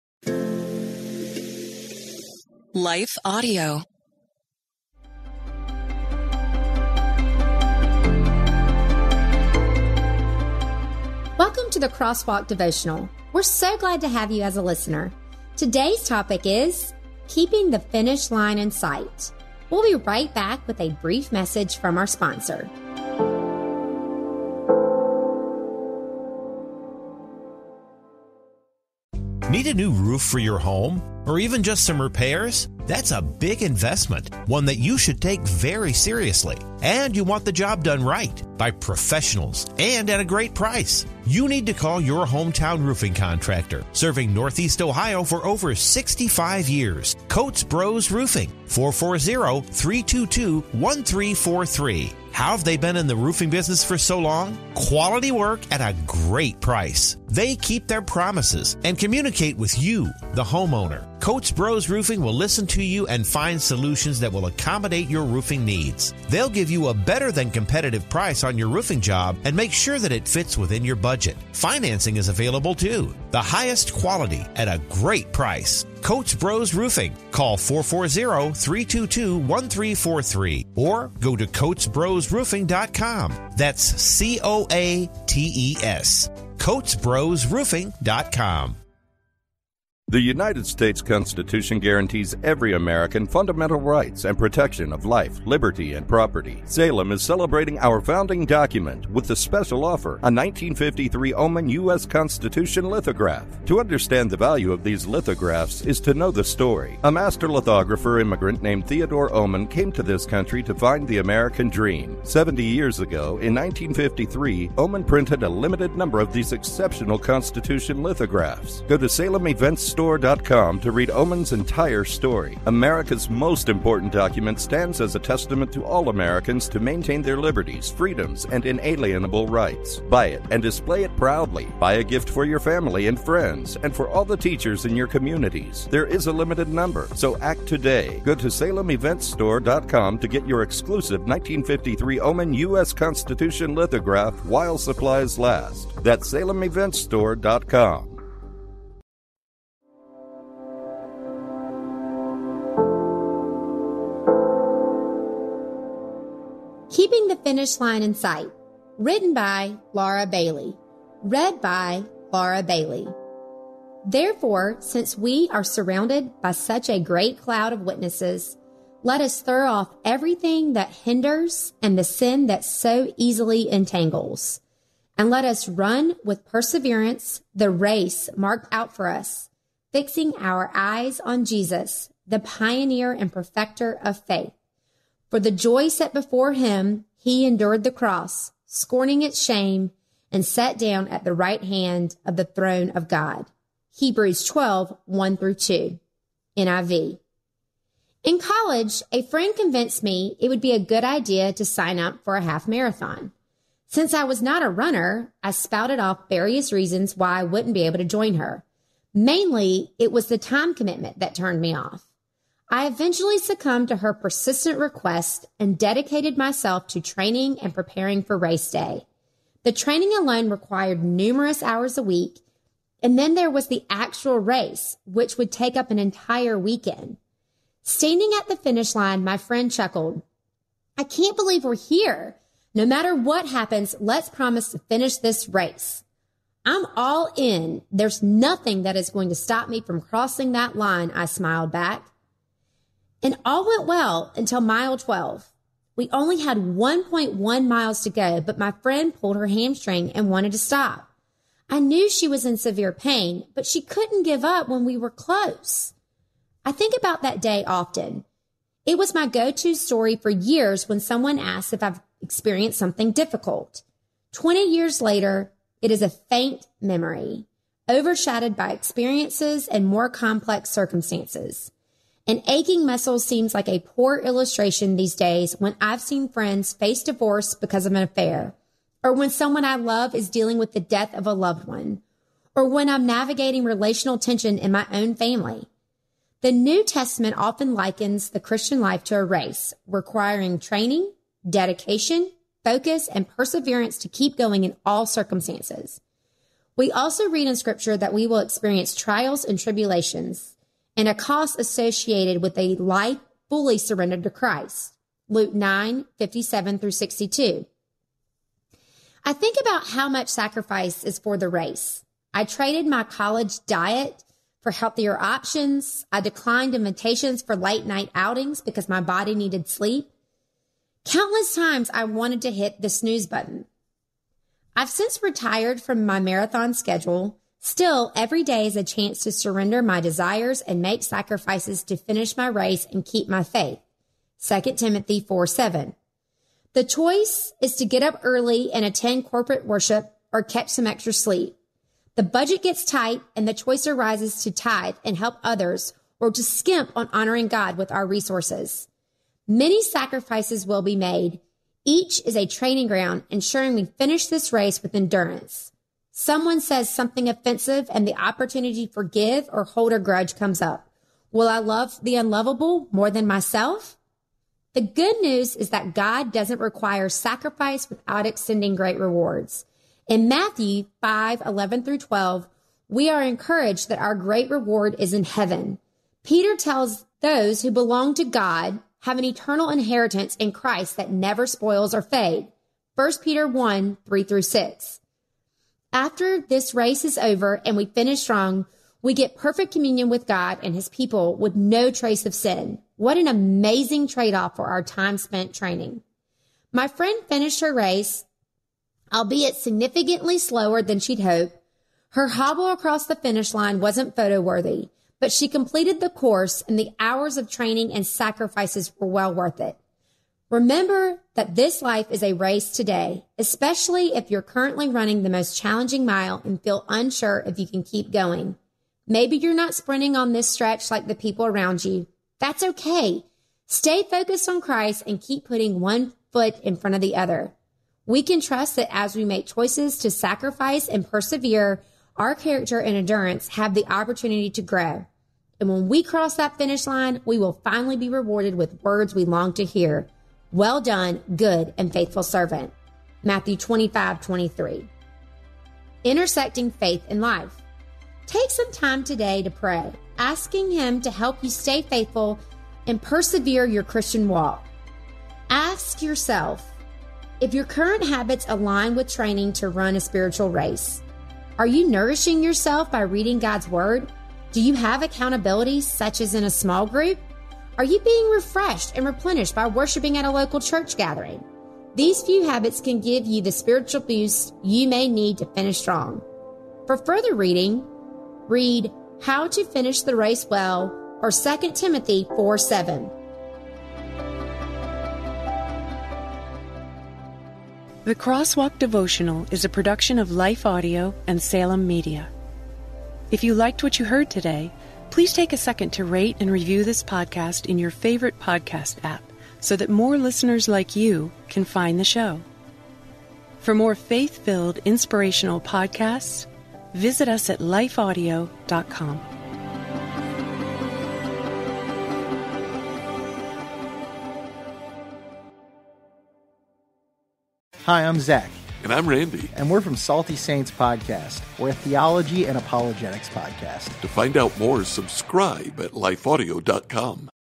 Keeping the Finish Line in Sight
Remember that this life is a race today, especially if you're currently running the most challenging mile and feel unsure if you can keep going. Maybe you're not sprinting on this stretch like the people around you. That's okay, stay focused on Christ and keep putting one foot in front of the other. We can trust that as we make choices to sacrifice and persevere, our character and endurance have the opportunity to grow. - Laura BaileySUBSCRIBE to our sister podcasts:Your Daily Prayer: Daily Bible Verse: Discover more Christian podcasts at lifeaudio.com and inquire about advertising opportunities